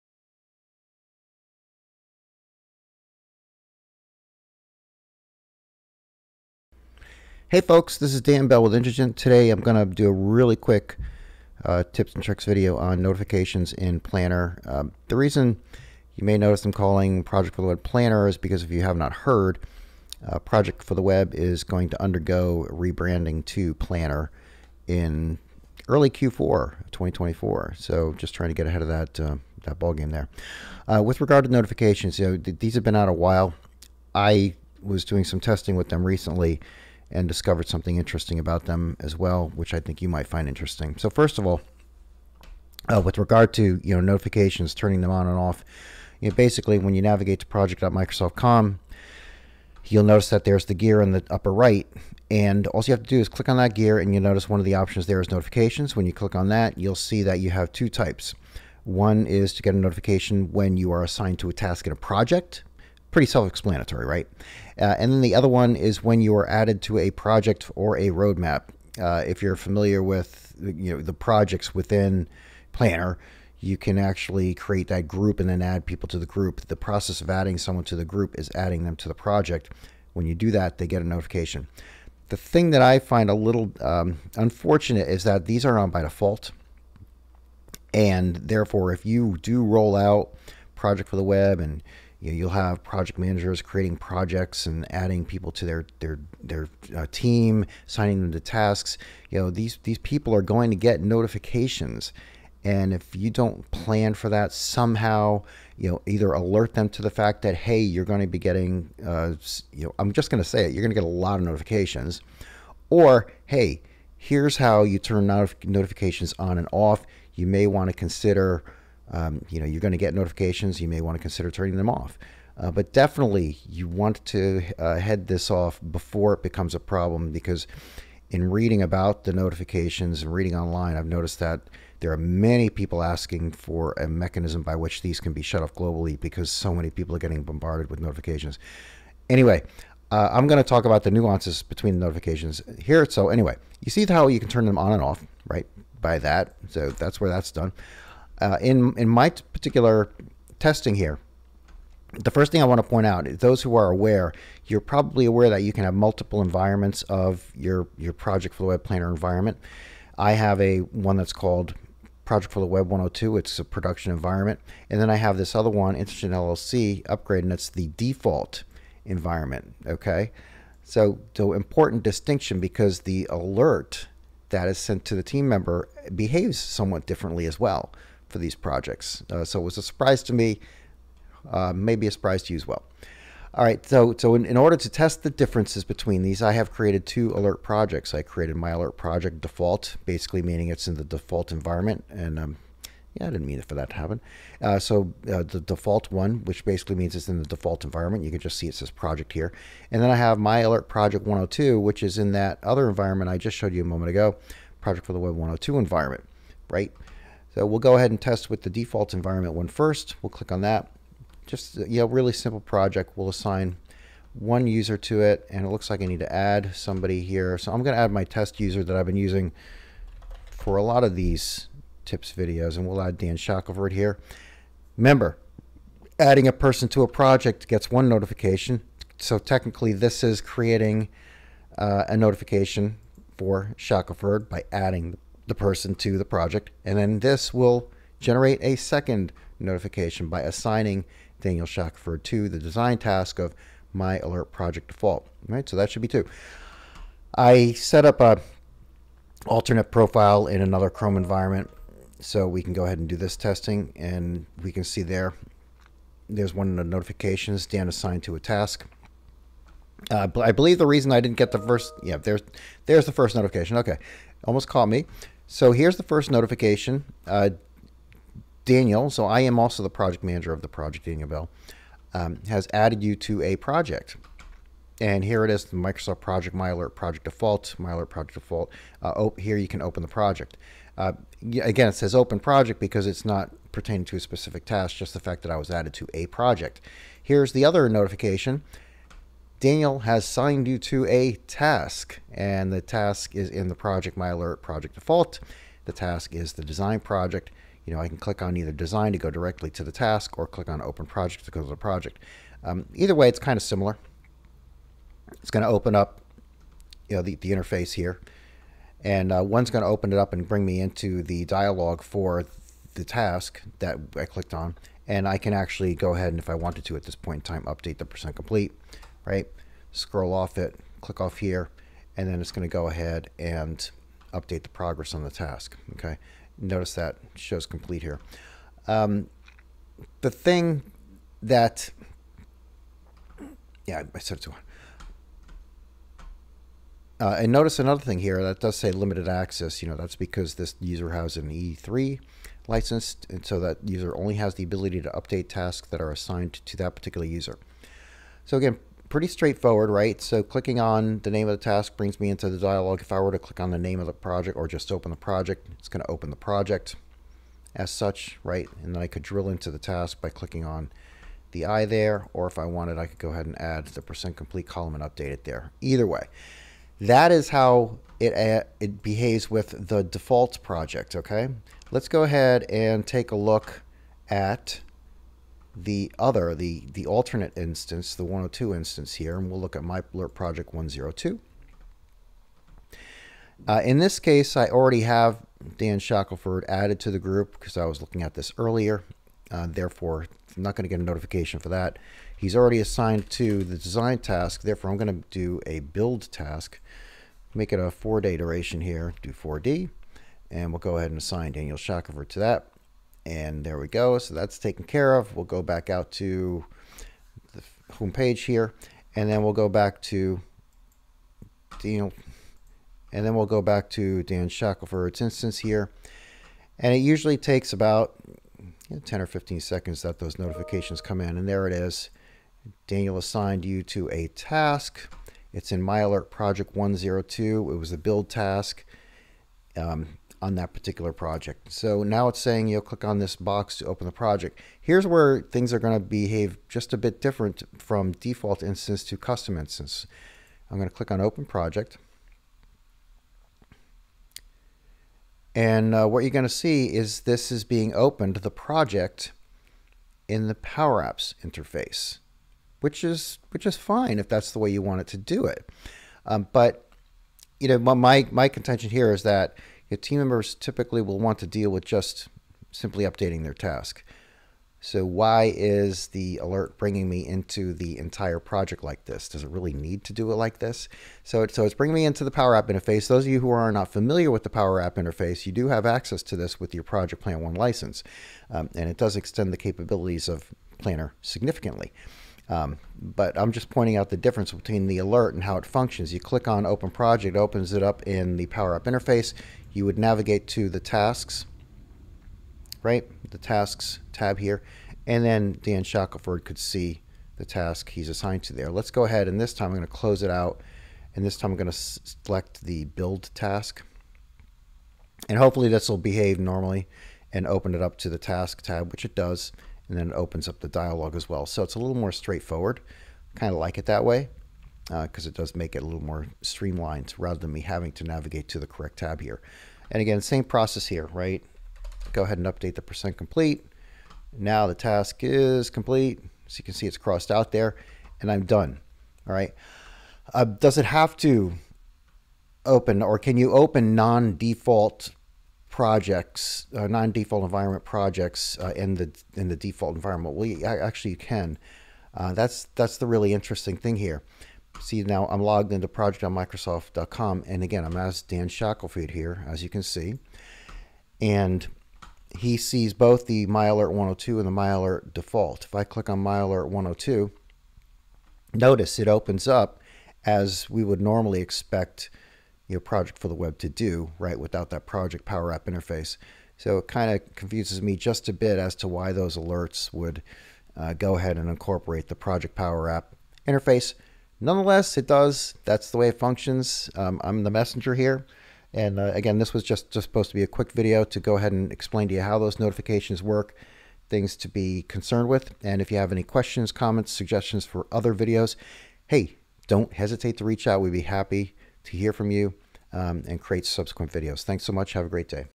Notifications in Project for the Web
Recently I was working with Project for the Web to determine how notifications functioned.  Email Notifications are quite often used in Project Management situations to keep team members and other stakeholders aware of important events.

In any event, the notifications are present and they work.  Let's hope Microsoft provides some global mechanism to turn them on or off.  Or at the very least, have them off by default!

Well certainly Project for the Web does have a notifications feature.  And notifications are sent to team members and others when they are added to a project, as well as when they are assigned a task.  I also learned that notifications are turned on by default.  I am not a fan of this setting, as people will get hit with quite a few emails upon deployment of Project for the Web unnecessarily.  Perhaps Microsoft will update this in the future.

Nonetheless, in my testing I noted that the email notifications behaved different depending on whether the Project for the Web environment was the default environment or not.  When the project was located in the default environment, the notification when clicked would take the team member to the expected project in the expected interface.

Conversely, I then created a new project and tested in the non-default environment, clicking on the link in the email notification would bring the team member to the project, however, this would be via the Project Power App interface.  This interface exposes the Team Member to quite a bit of information they have no need to see or interact with.  My concern being this is not as streamlined as I would like it to be.

Please watch this video to see the results of my testing!